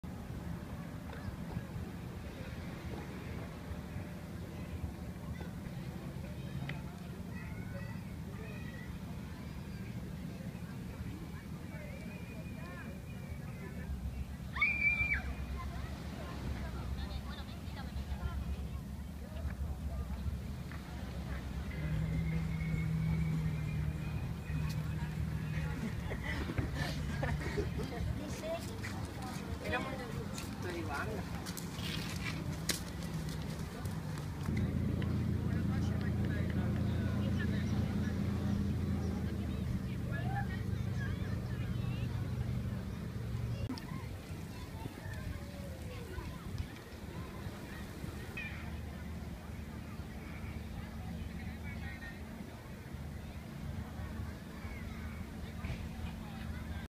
A day at the beach, Sabinillas, Manilva. Spain
A warm, calm and beautiful day at Sabinillas beach in Andalusia, Spain